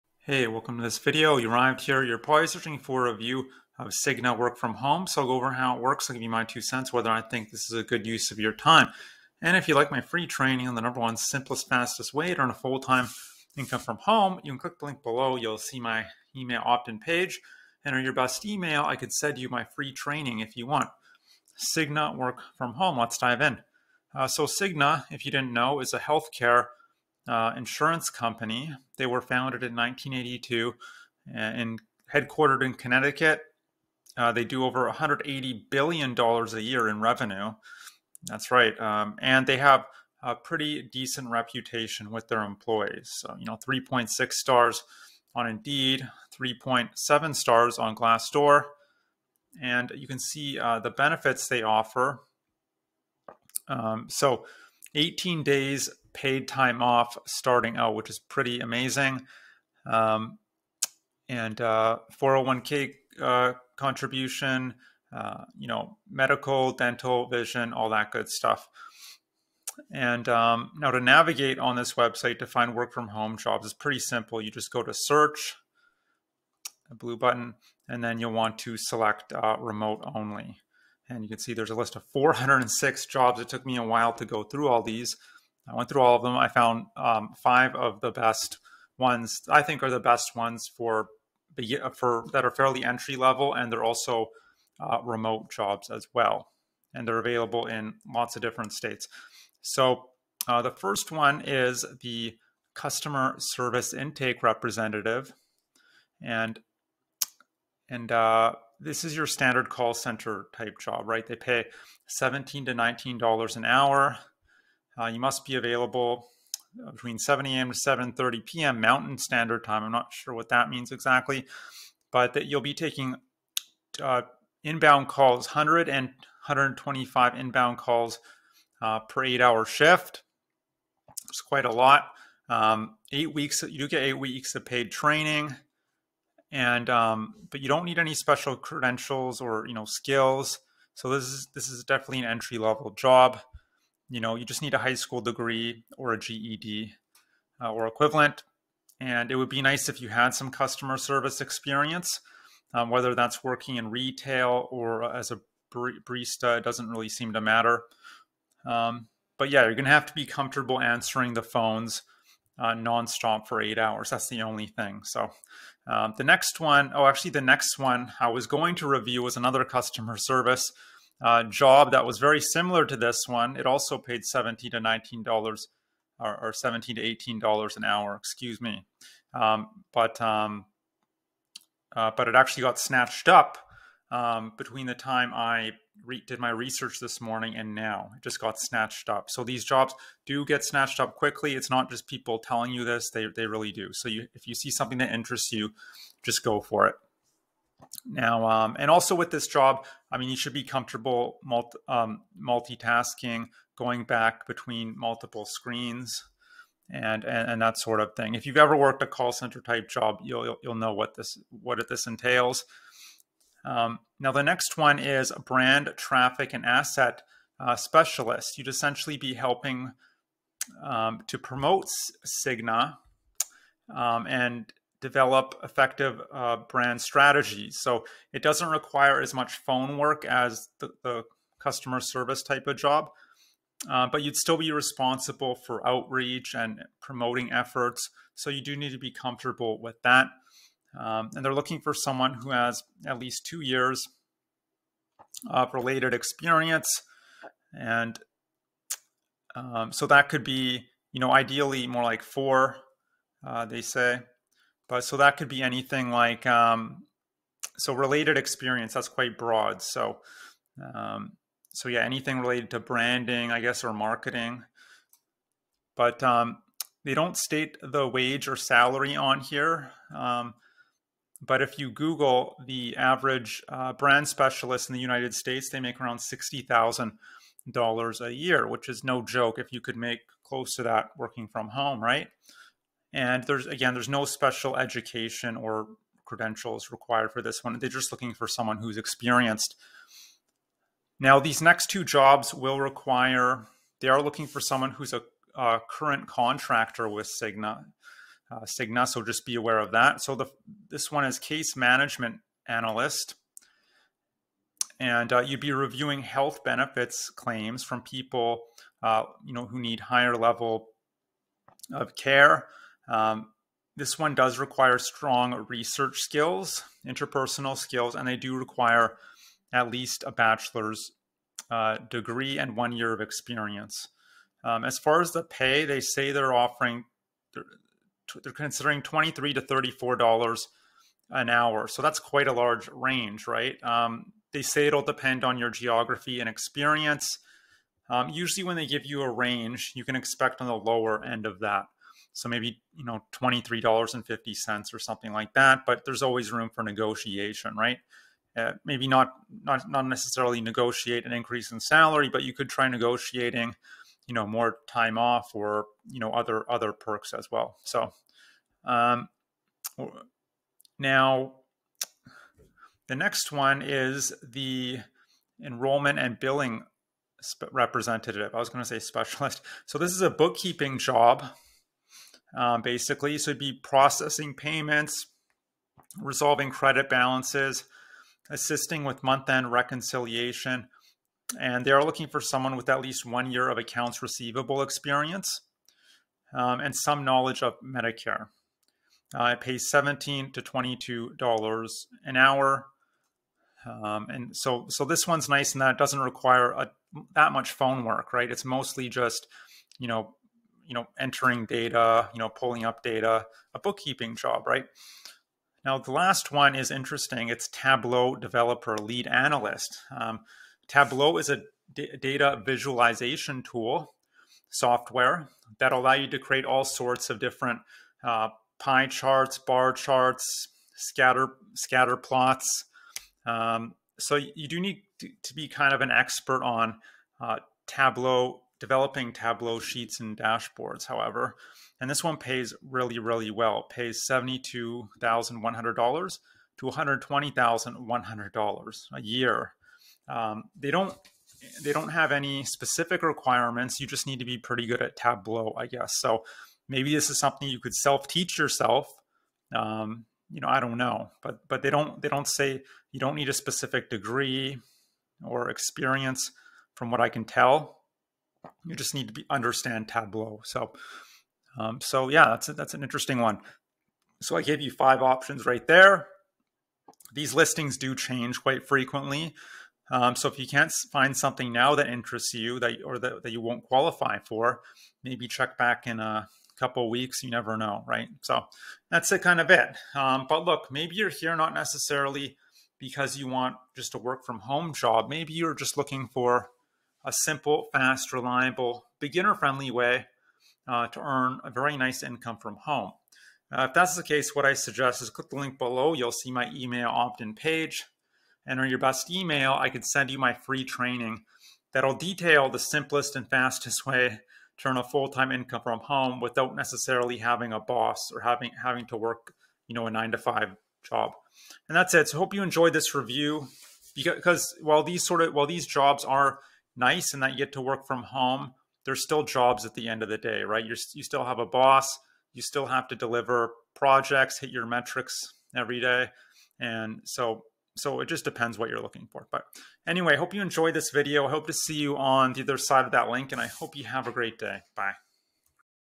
Cigna Work From Home Review - Do They Have Any Non-Phone Jobs?
Cigna Work From Home Review?

About this video:
In this Cigna Work From Home review you will learn all about this healthcare insurance company and you should apply to some of their remote jobs. Most of these types of opportunities are not that great so watch this review and let me know what you think.